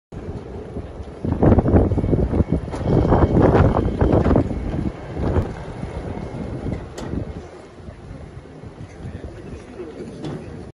Робот "Маркер"
Дрон патрулирует территорию космодрома "Восточный"